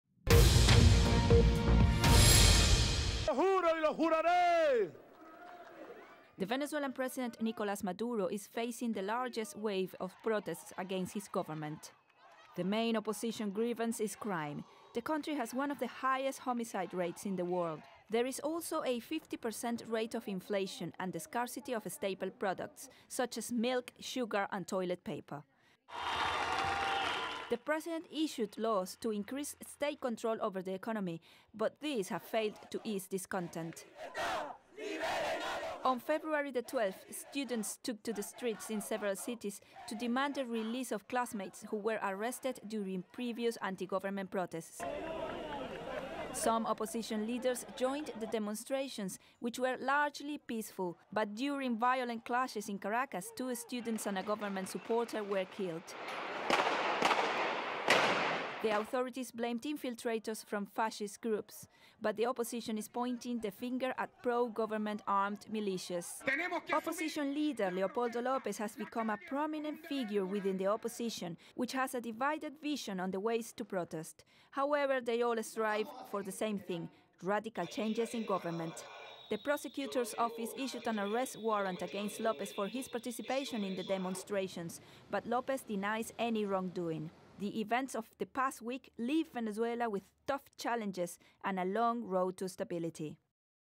Venezuela Crisis explained in 90 seconds - BBC NEWS
Rallies across Venezuela have been taking place both for and against President Nicolas Maduro. The protests have been going on since last week and the opposition leader Leopoldo Lopez has handed himself into police after he was wanted after being accused of inciting violence.